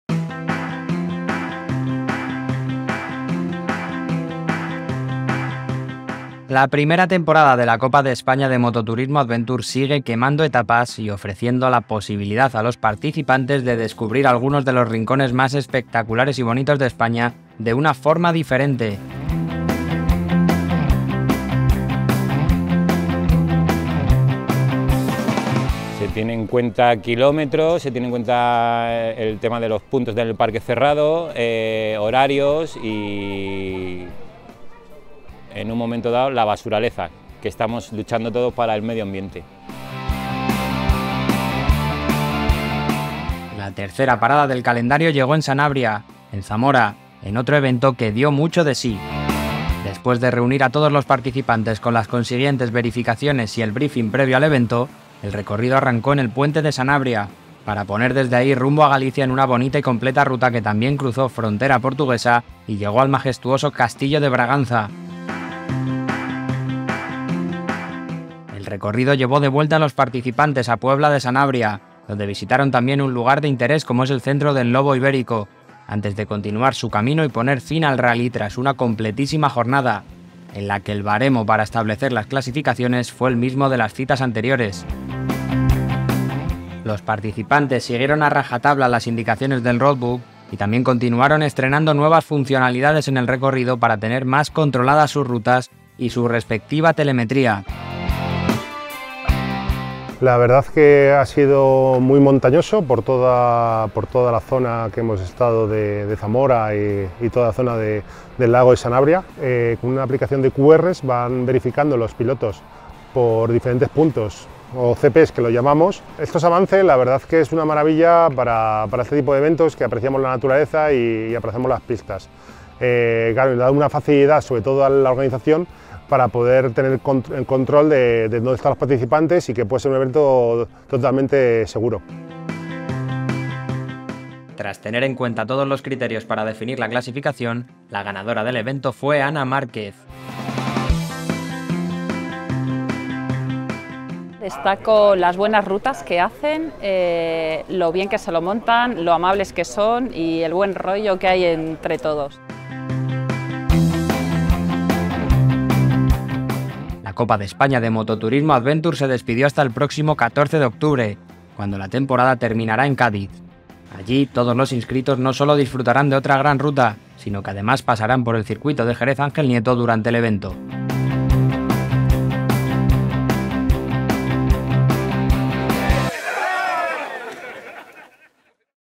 Mototurismo Adventure Sanabria 2022
La tercera cita de la Copa de España de Mototurismo Adventure (CEMA) tuvo lugar el pasado fin de semana con un espectacular recorrido que atravesaba las provincias de Orense, León y Zamora.